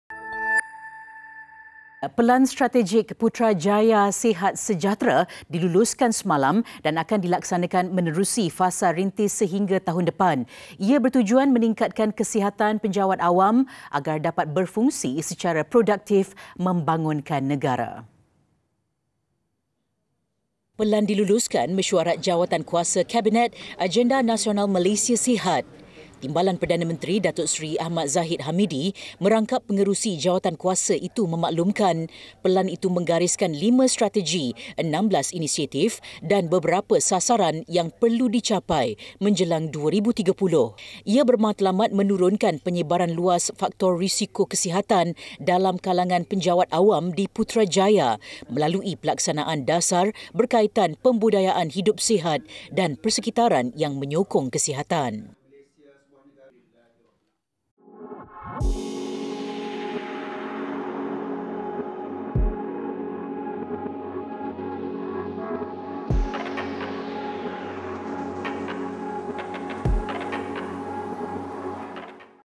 PENJAWAT AWAM | Pelan Strategik Putrajaya Sihat Sejahtera Diluluskan
Pelan strategik Putrajaya Sihat Sejahtera diluluskan semalam dan akan dilaksanakan menerusi fasa rintis sehingga tahun depan.